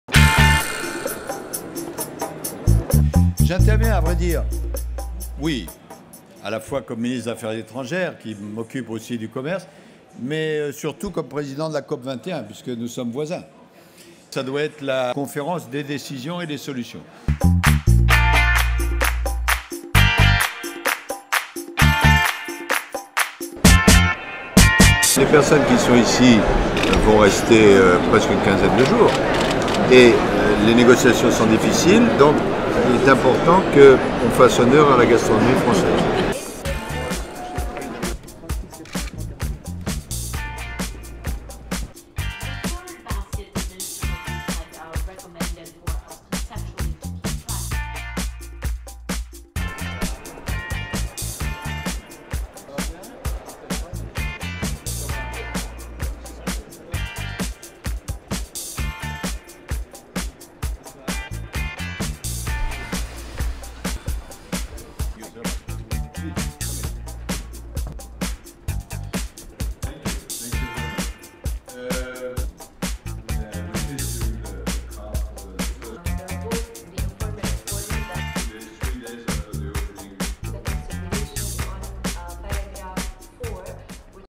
Le Président de la COP21 en action : mercredi 2 décembre
Suivez l'action du président de la COP21 au jour le jour.

Instragram : @francediplo